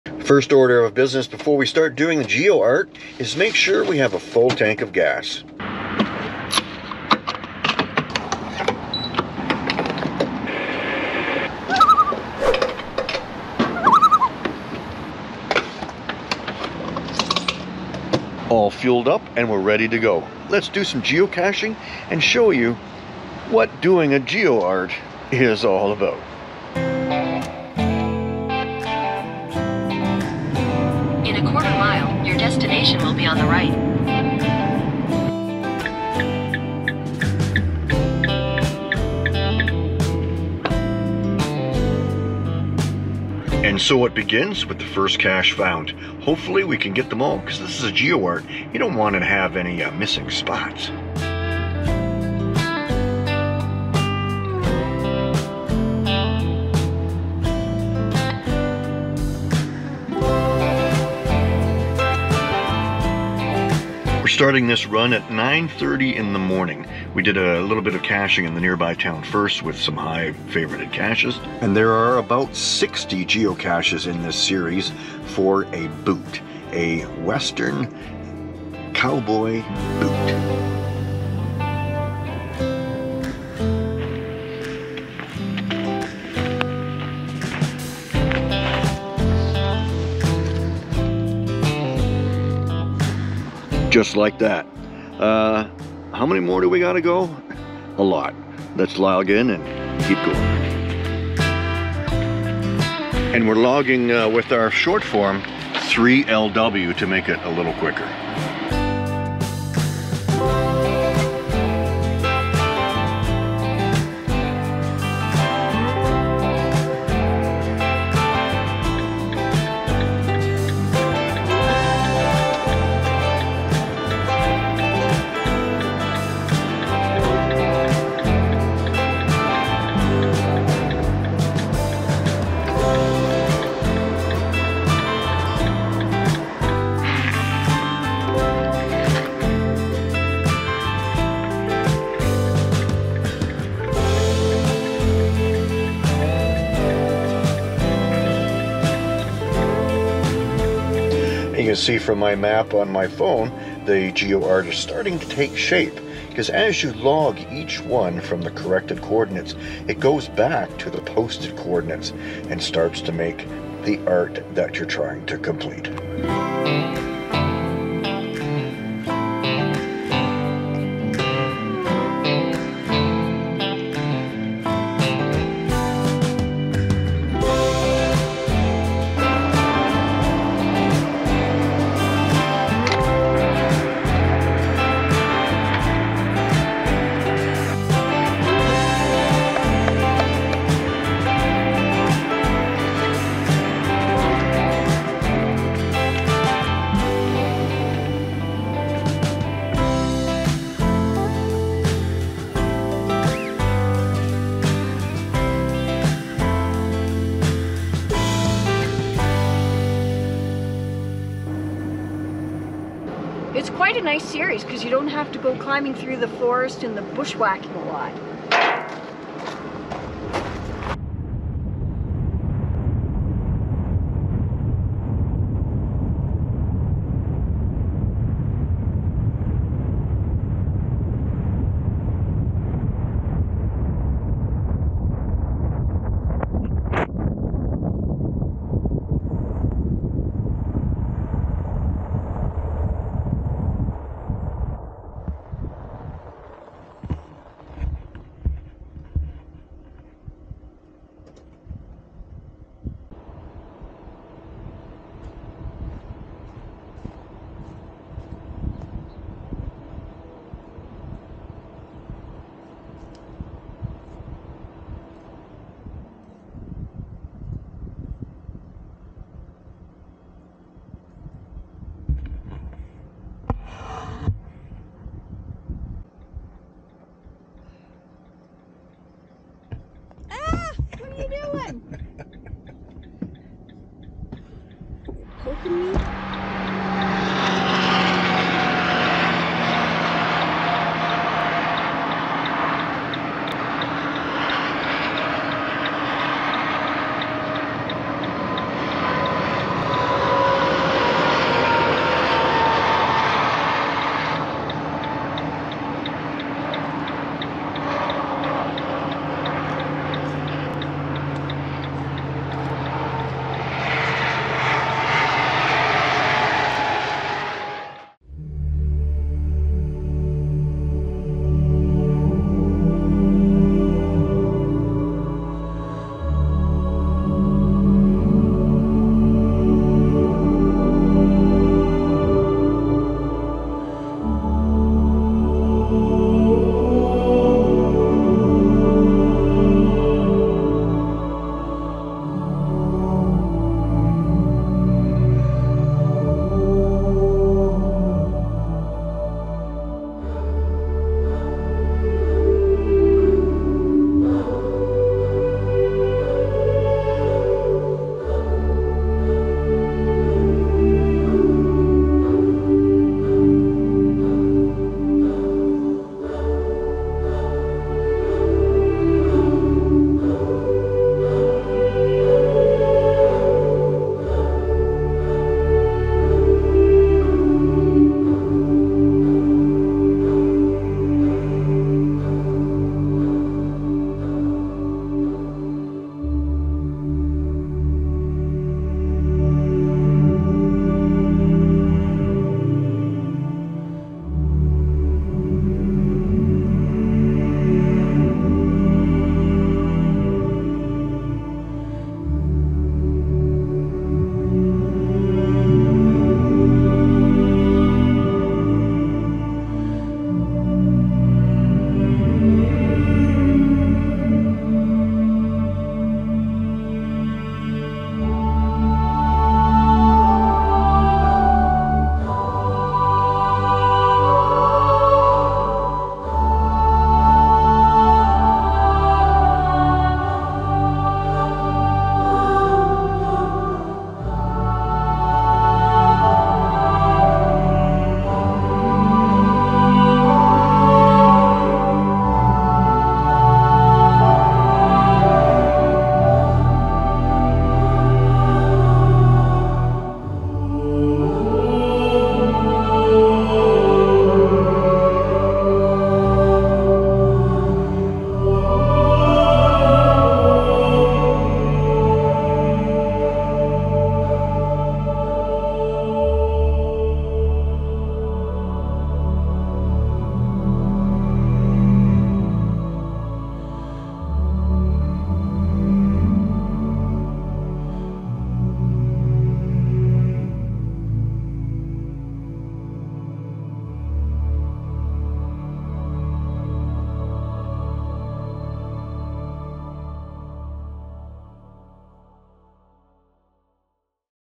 Geocaching for Geo-art
What is it like to do a Geo-art? There is a lot of driving back roads, side roads or walking trails and finding cache after cache after cache, the same type, size and condition. The effort put in is worth the satisfaction of seeing the art made up of smilies.